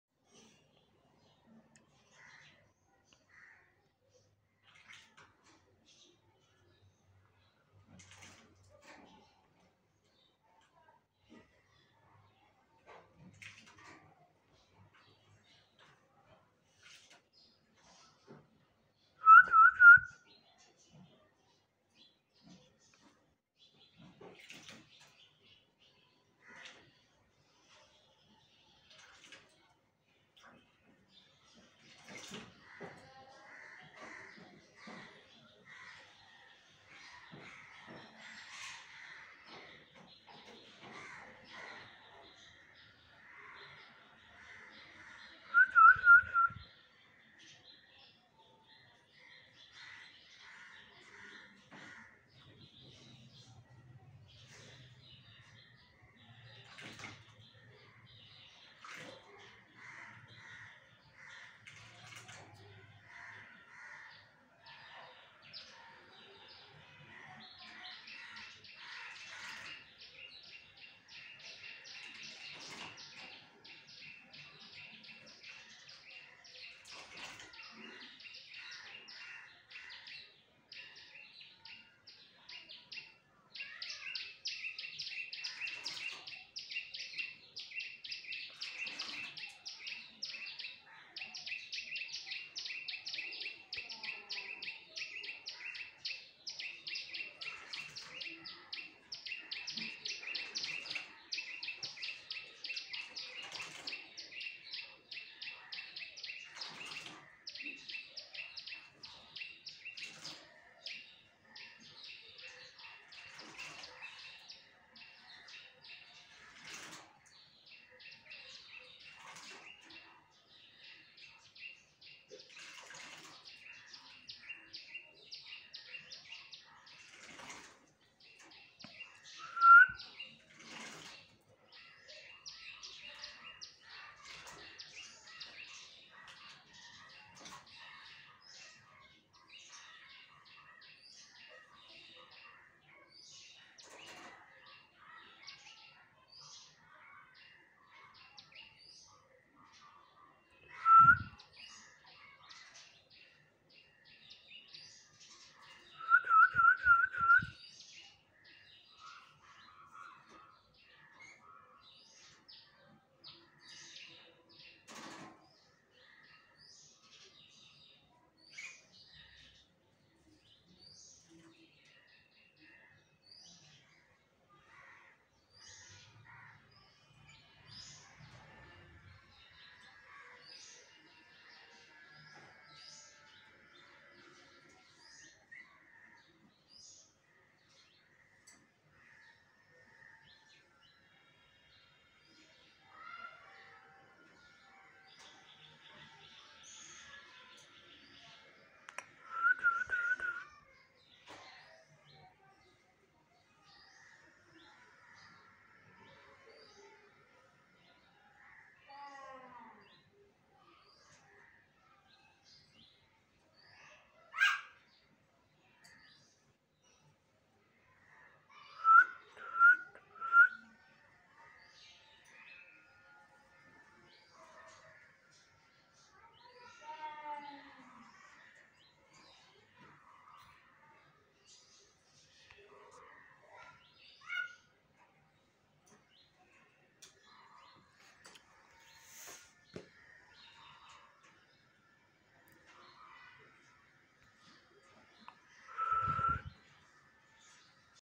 শালিক পাখির গোসলের ভিডিও ভাইরাল/ kotha bola pakhi salik সালিক_পাখি #salikh
Talking Bird

Bangladeshi local bird talking and friendly behave with human

জুট শালিক এবং ভাত শালিকের মধ্যে কথা বলার প্রতিযোগিতা ।দেখেন কোন পাখি ভাল কথা বলে। shalik pakhi